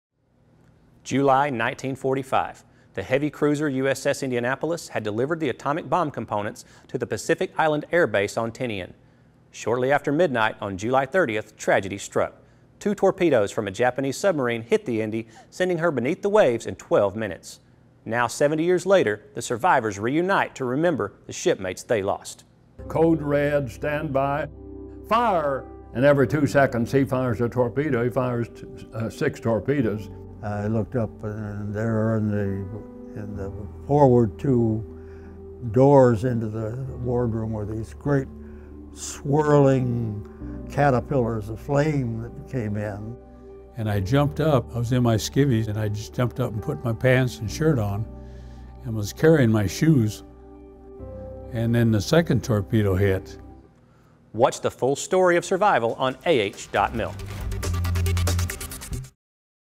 USS Indianapolis Survivors Reunite
Sailors tell their stories of survival after their ship was torpedoed and sank in the Pacific in 1945.